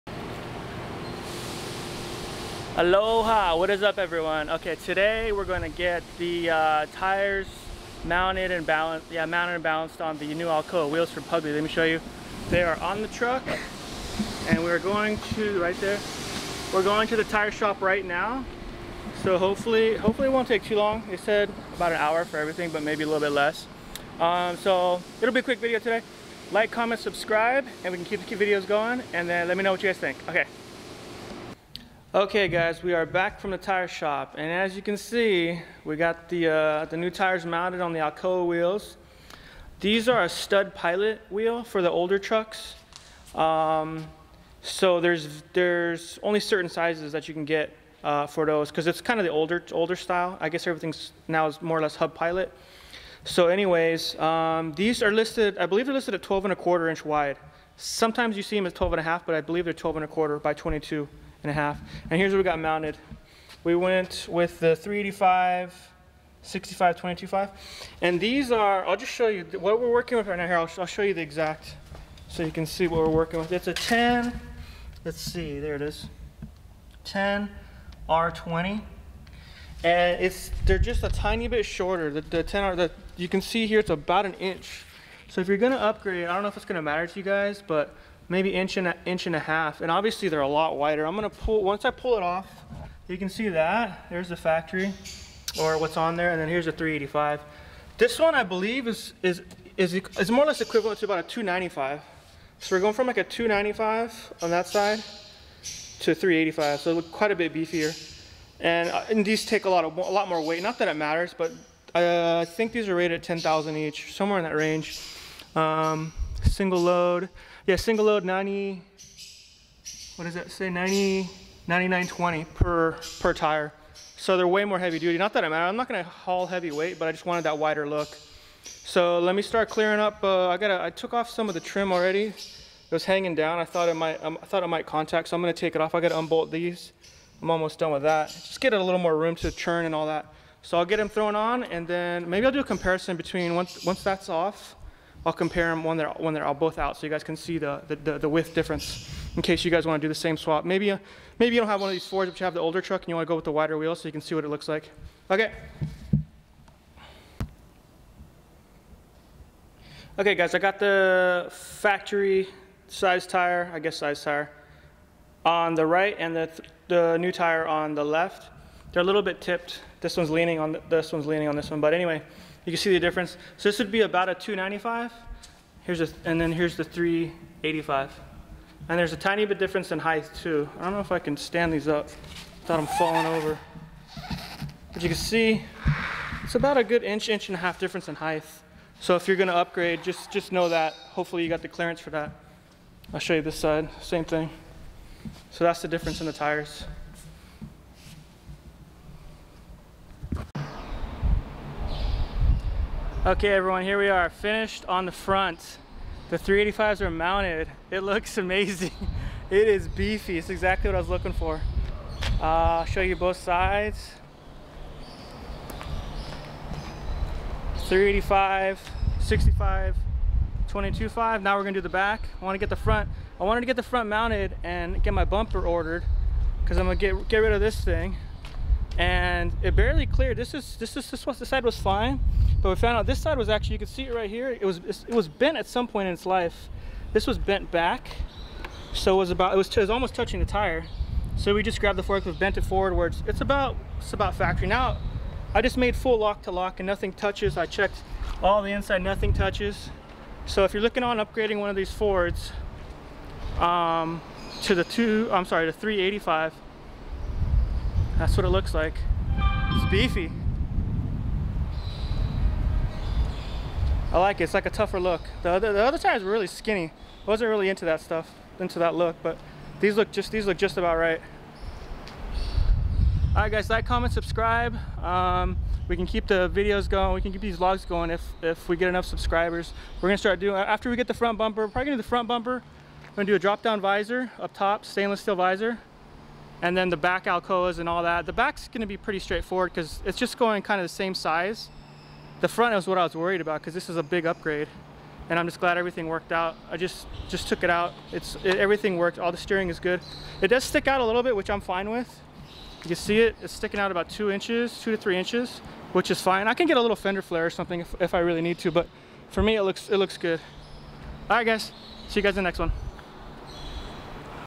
Project Pugly: Alcoas on 385s DONE!!
Project Pugly finally getting his new front wheels and tires! Alcoa 12.25x22.5" wheels wrapped in 385/65/22.5 tires.